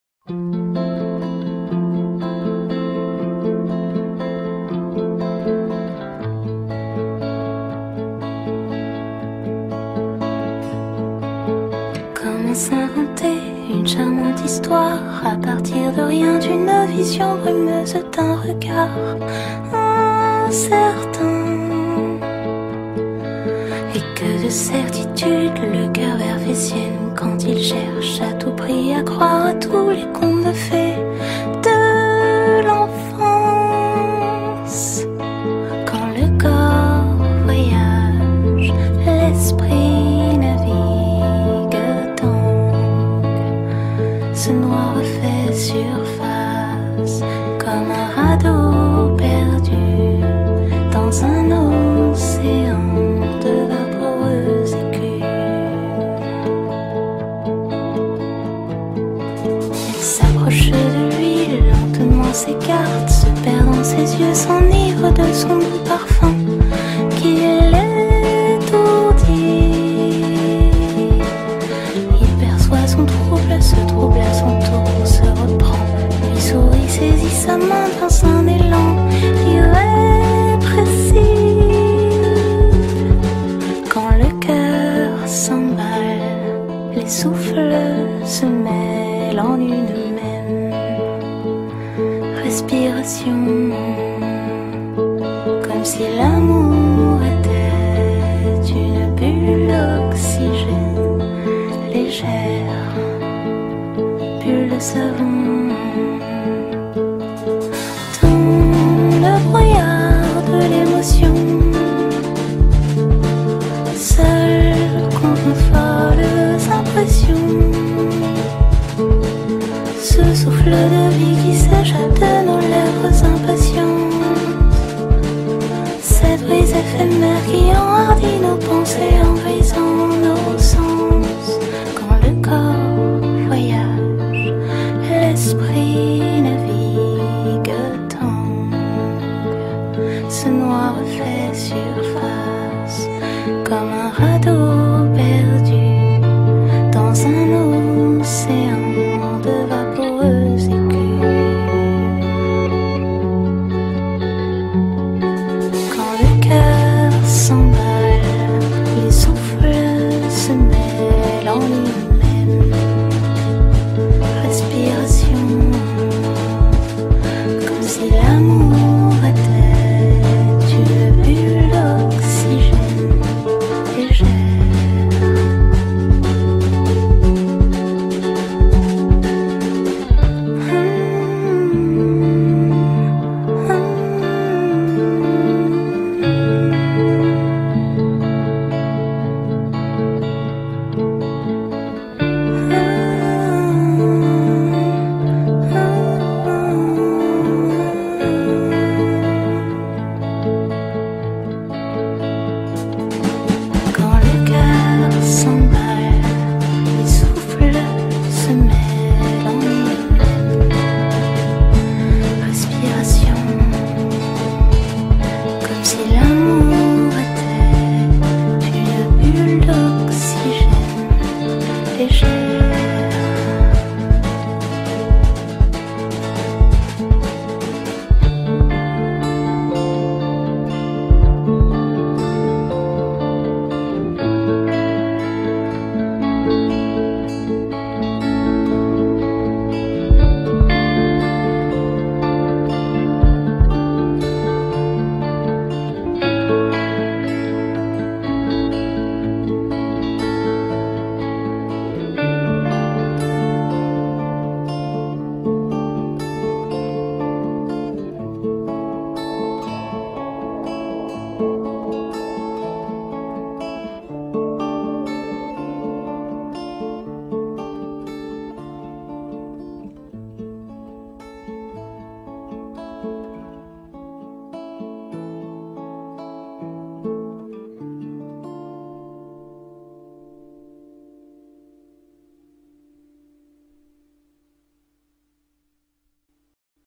L'ESSENTIALISME, Jose Garcia PELUQUEROS
Esta colección de Jose Garcia PELUQUEROS está inspirada en la capital francesa a finales de los años 30, L'ESSENTIALISME, está centrada en esas mujeres, que sin nada de valor material, tienen que buscarse la vida y aprovecharse de algo que nadie les podrá arrebatar nunca; el esencialismo.